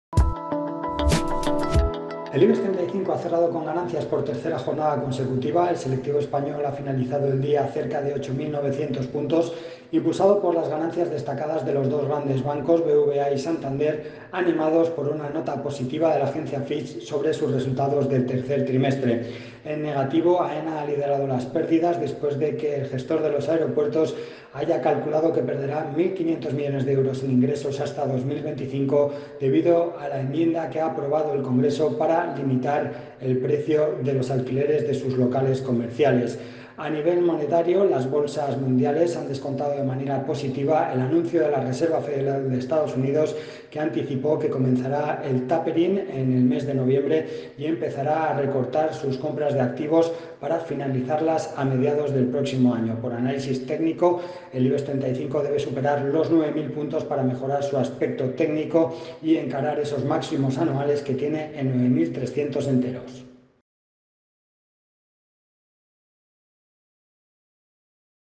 El Ibex extiende su rebote y roza los 8 900 puntos impulsado por BBVA y Santander
El Ibex 35 ha extendido su rebote de las últimas jornadas tras subir un 0,78%, hasta 8.876,90 puntos, en una jornada alcista en Europa después de las alzas que se vieron ayer en Wall Street (Dow Jones: +1%; S&P: +0,95%; Nasdaq: +1,02%) tras confirmar la Reserva Federal (Fed) que "pronto" comenzará a reducir su programa de compras de activos, un proceso conocido como 'tapering'. Los dos grandes bancos, Santander y BBVA, han sido los principales motores del selectivo español, tras una nota positiva de Fitch que ha sido destacada por Bolsamanía.